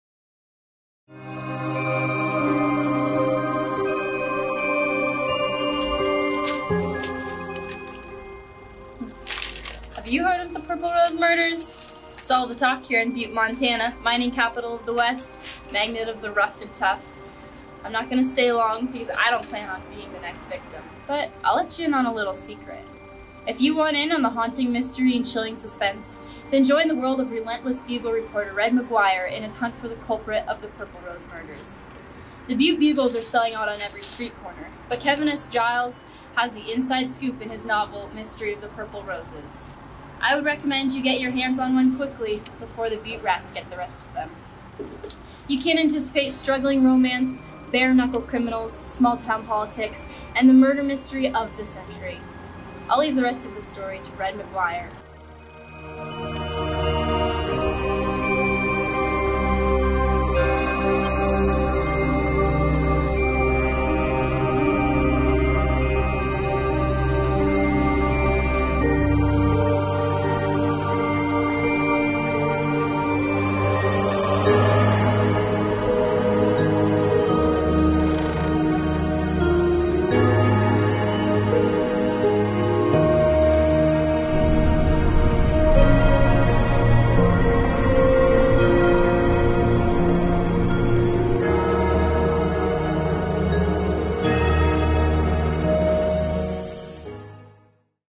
Mystery of the Purple Roses, a novel of dark streets and shady characters
In this hard-boiled crime detective novel, a series of curious murders haunt Kieran “Red” Maguire, crime reporter for the fictional Bugle daily newspaper, through the summer of 1954 in Butte, Montana. They appear to have nothing in common with one another except for a single purple rose placed on each victim. Red and his hard-nosed crime-fighting accomplice, police captain Harold “Duke” Ferndale, struggle to find clues that will solve a chilling mystery that terrifies residents of the Mining City. Red, a love-deprived but handsome Irishman, encounters adoring women, bare-knuckle criminals, outrageous characters who roam the streets, and a relentless Bugle editor who spares no murderous details so newsboys can bark lurid headlines in uptown bars. Through it all, Red copes with haunting memories of his pretty young mother, reminding the reader that no hero escapes the clutches of past regrets. Sold at Booklocker.com, Amazon.com and other major online book retailers.